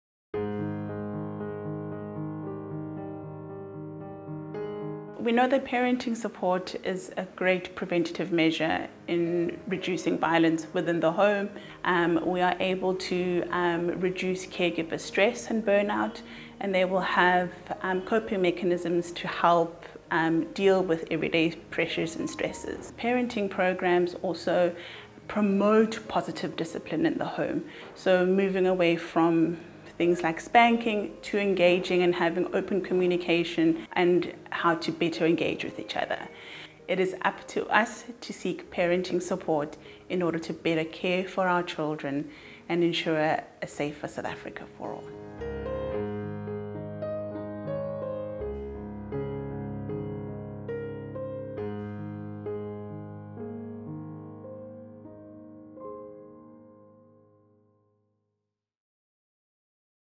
How parenting support reduces stress and prevents violence
Gloria Khoza, Child Protection Officer at UNICEF South Africa, speaks to us about how parenting support interventions and programmes prevent violence in communities.

Raising a child?  Find the support that works for you.

or WhatsApp ParentText on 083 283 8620